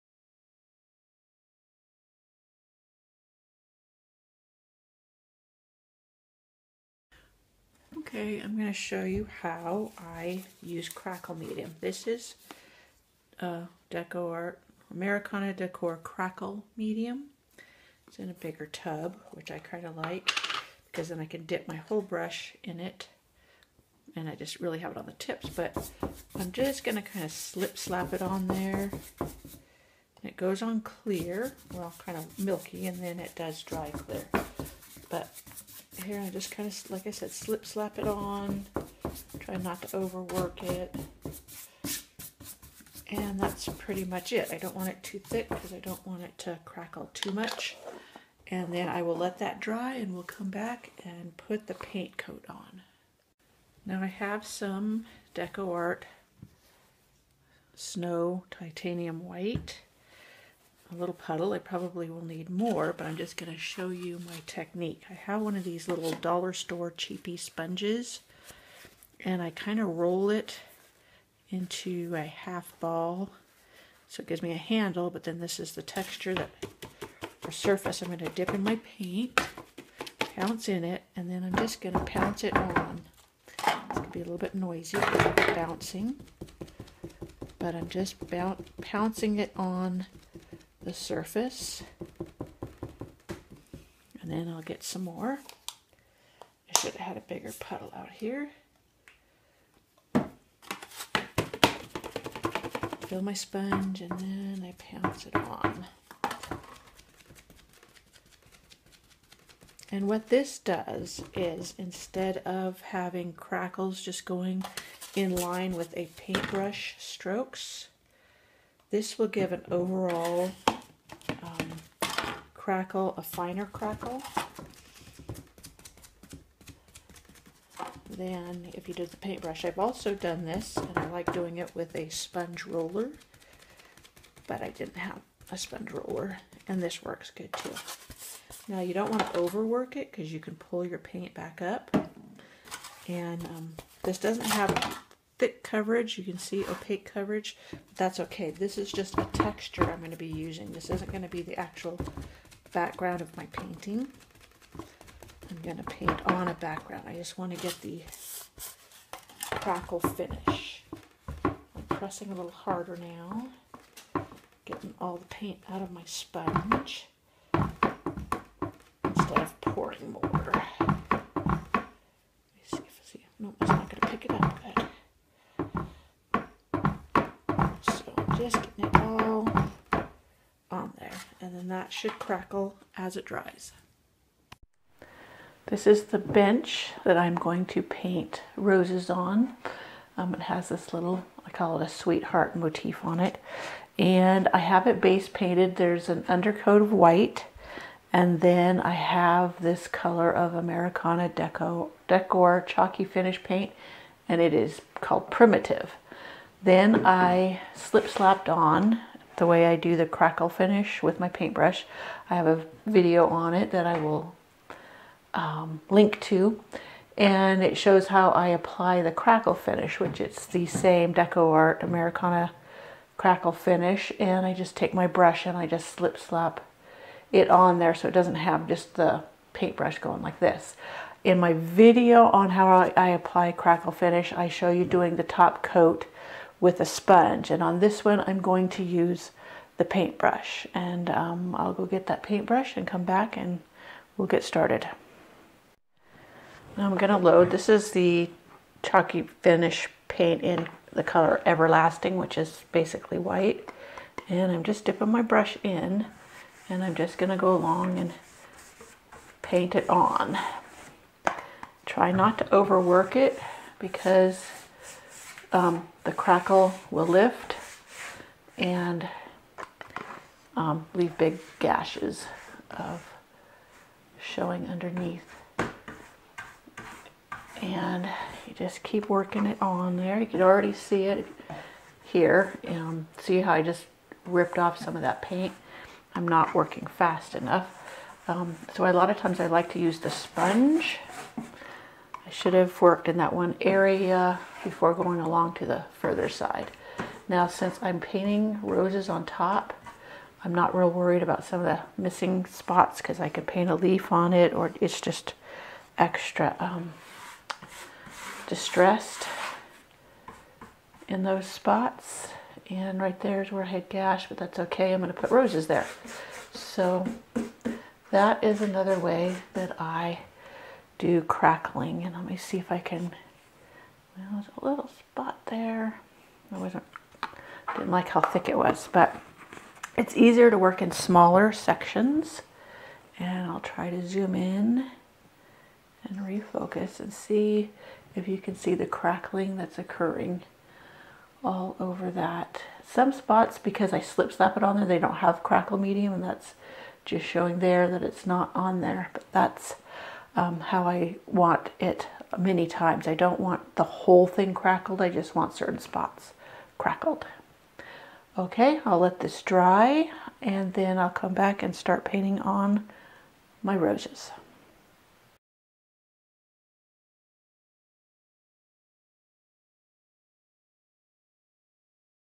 How to Use Crackle Mediums
Crackle mediums can add such a fun dimension to your painting surface.  Come see how I apply crackle medium and get the effect I want every time.